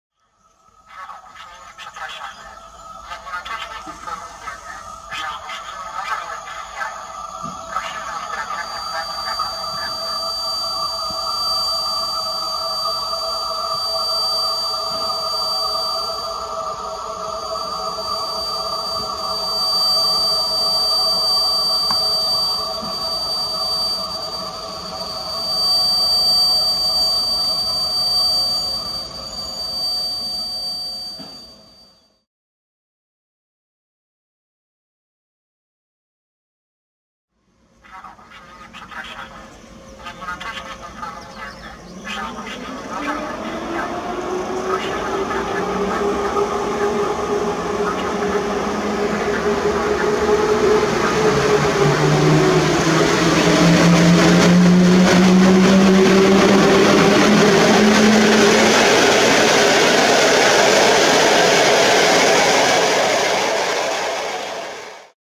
EP07-330 Zielony Trapez z TLK 5600/1 Bursztyn do Wrocławia Głównego.
EP07-330 Zielony Trapez z krótkim 4 wagonowym składem pociągu TLK 5600/1 Bursztyn z Gdyni Głównej do Wrocławia Głównego na stacji Żmigród.

Województwo: Dolnośląskie
Miejscowość: Żmigród
Data: 23 Czerwiec 2016r.
Godzina: 13:55
Przewoźnik: PKP Intercity
D29: 271 (Wrocław - Poznań)
Montaż: Camtasia Studio